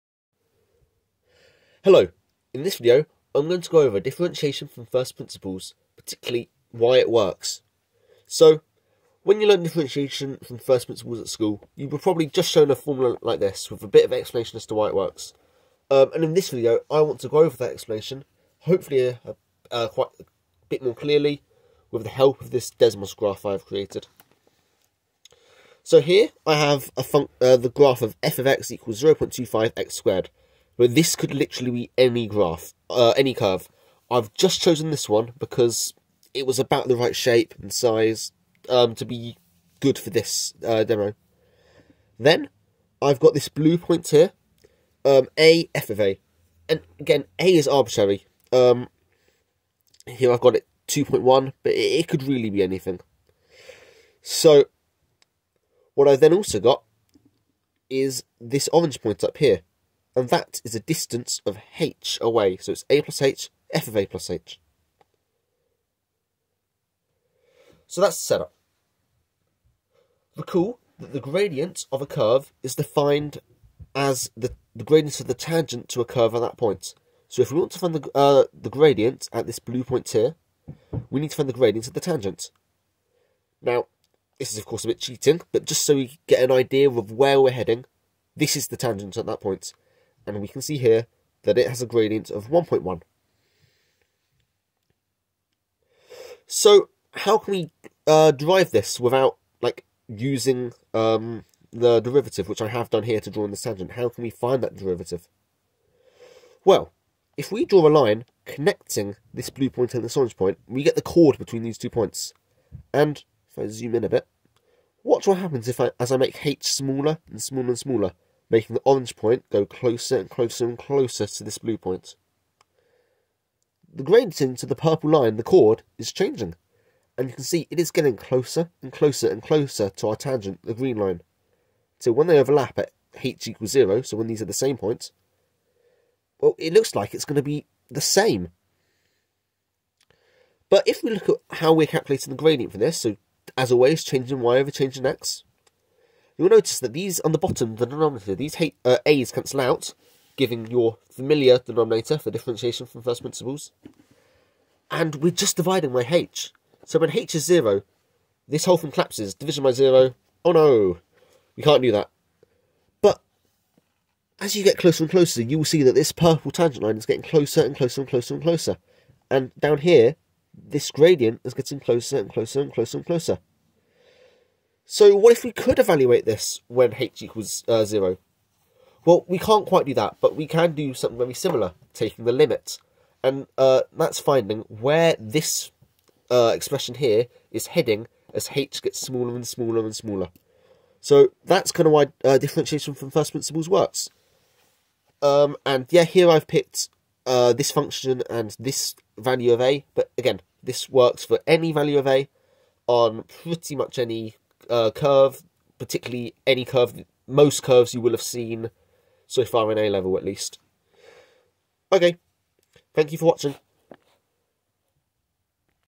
Why differentiation from first principles works
In this video I go over why differentiation from first principles works using a Desmos graph that I made.